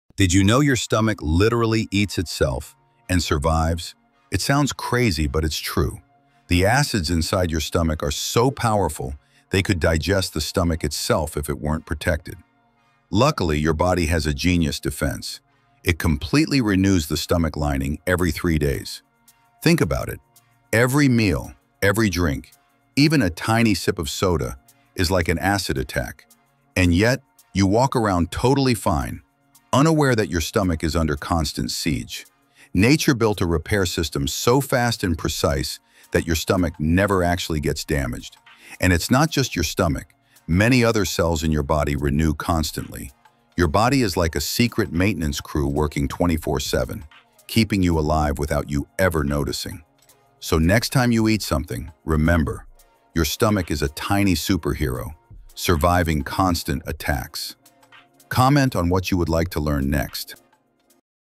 The Stomach's Superhero Defense System
Your stomach is literally eating itself right now... and somehow surviving!

This might sound like science fiction, but it's happening inside your body at this very moment. The hydrochloric acid in your stomach is so incredibly powerful that it could dissolve metal, yet your stomach manages to contain this corrosive liquid without being destroyed.

How is this possible? Your body has developed one of the most incredible defense mechanisms in nature - it completely replaces your entire stomach lining every 72 hours. That's right, every three days you get a brand new stomach lining!

Every time you eat a meal, drink coffee, or even take a sip of soda, your stomach unleashes a chemical attack that would be devastating to almost any other part of your body. But thanks to this amazing regeneration system, your stomach wins the battle every single time. # viral

But here's the really mind-blowing part - your stomach isn't the only overachiever in your body. Right now, millions of your cells are quietly renewing themselves, creating an invisible maintenance crew that works around the clock to keep you healthy and alive.

Your body is performing miracles every second, and you probably never even think about it. The next time you sit down for a meal, take a moment to appreciate the incredible biological superhero that is your stomach - constantly under attack, constantly rebuilding, and never giving up.

What other amazing body facts would you like to discover?